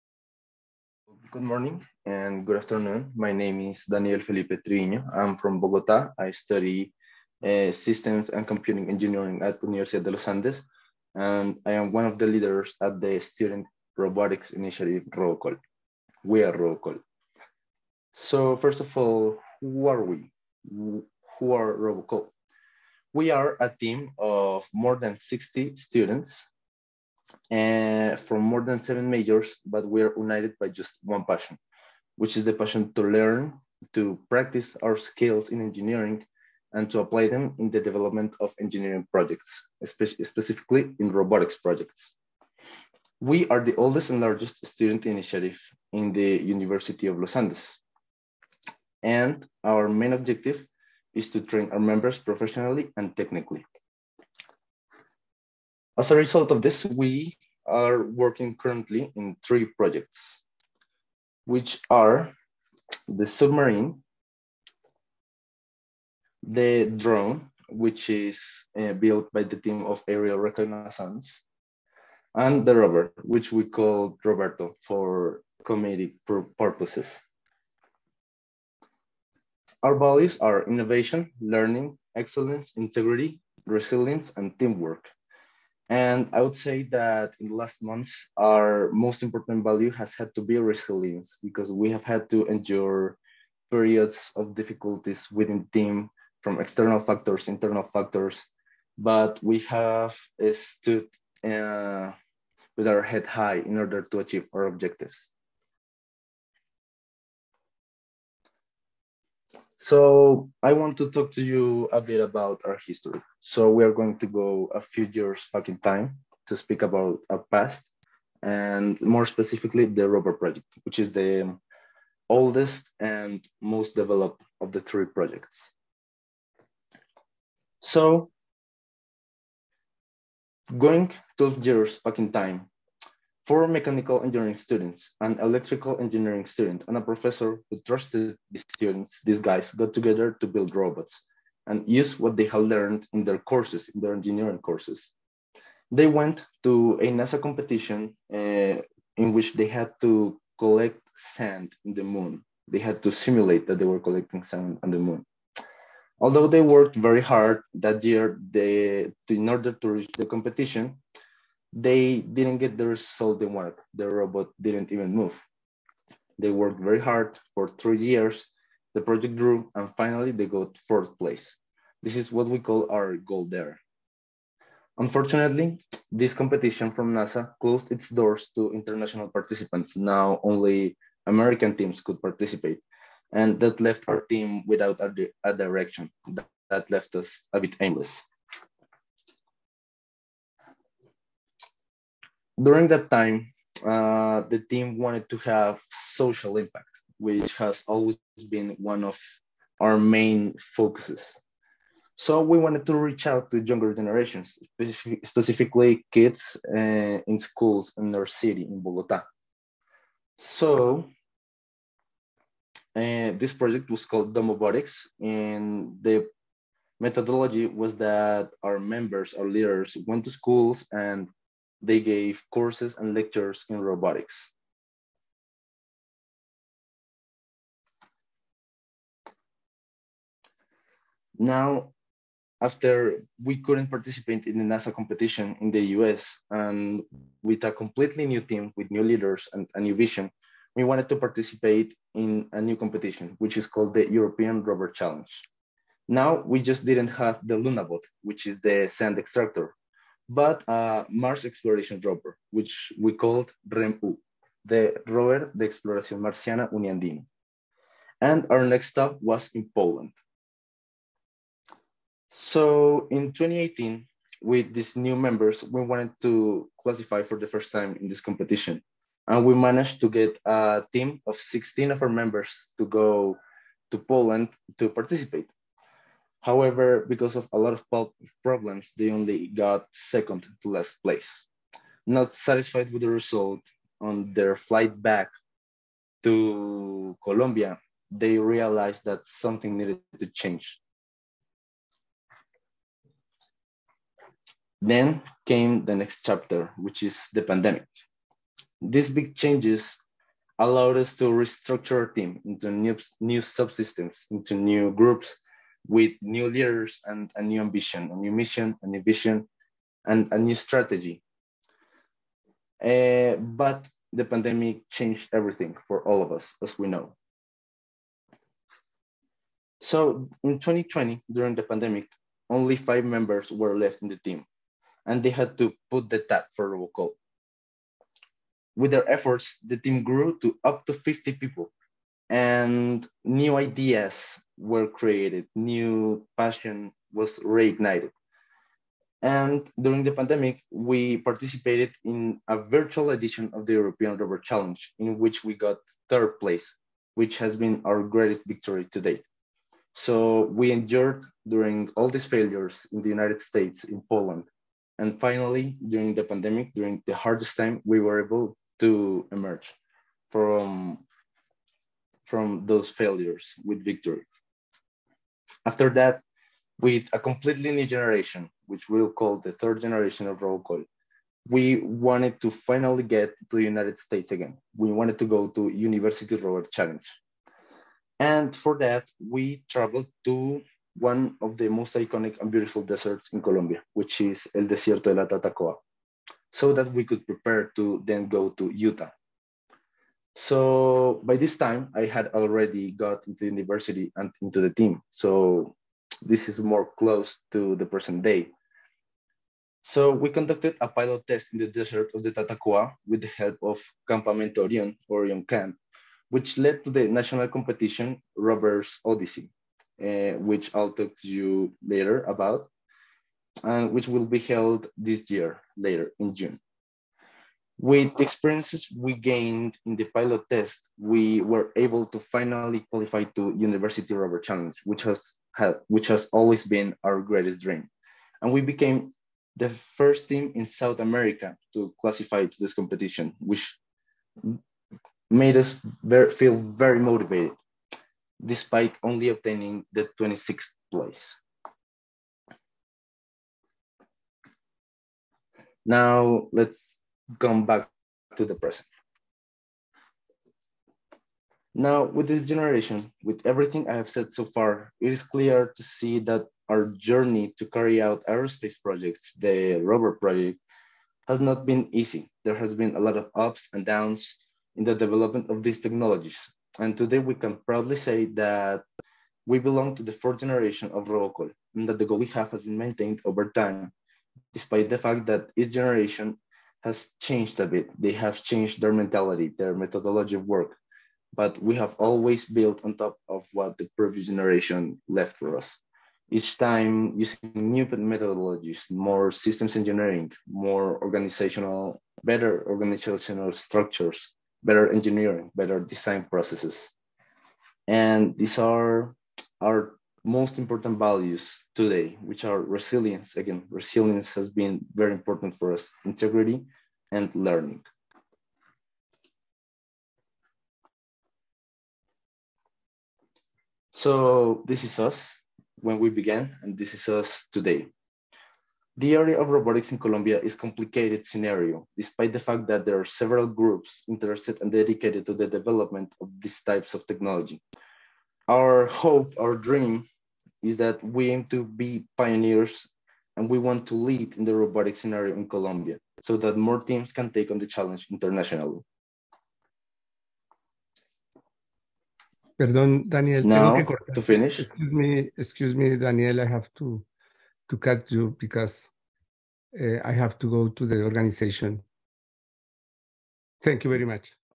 30th Virtual UNISEC-Global Meeting - ROBOCOL Program, students initiatives
Presentation Title: ROBOCOL Program, students initiatives
Presenter: Daniel Triviño, Universidad de los Andes, Bogotá

30th Virtual UNISEC-Global Meeting (February 18, 2023)